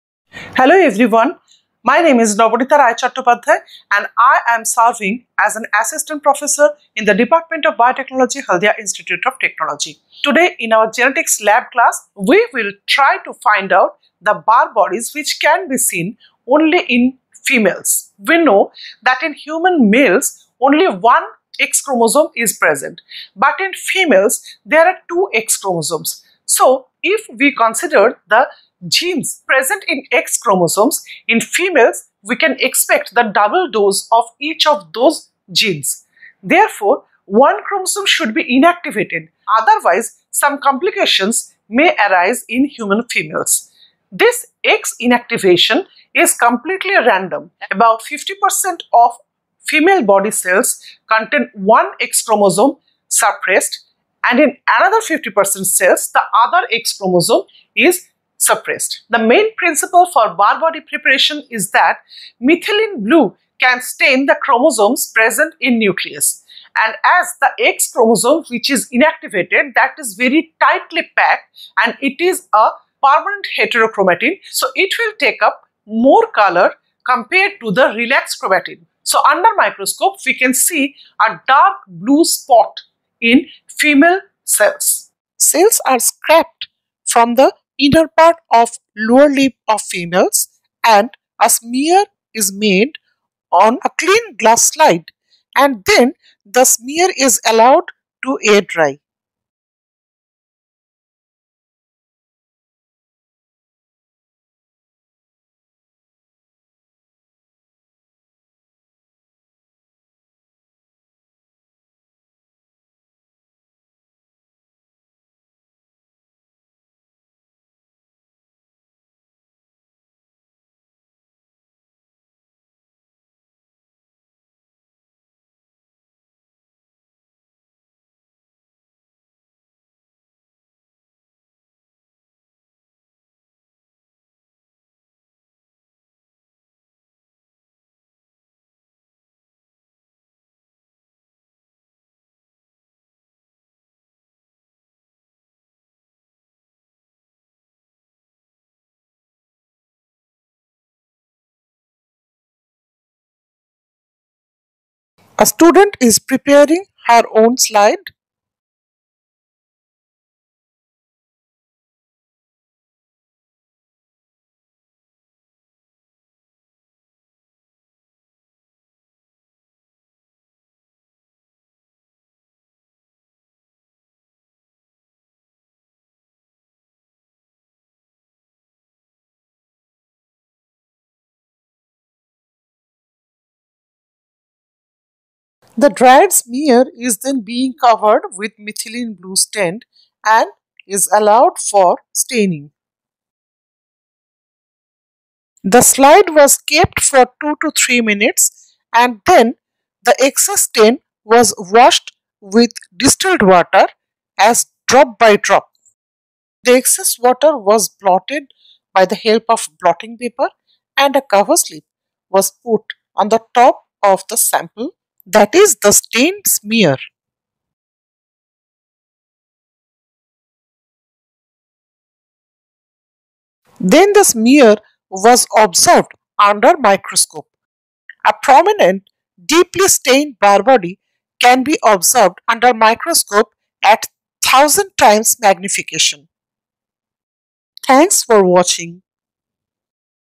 Barr body preparation from female buccal smear
Video Lecture Series by Dr. Nabanita Roy Chattopadhayay, Assistant Professor, Dept. of Bio-Technology, Haldia Institute of Technology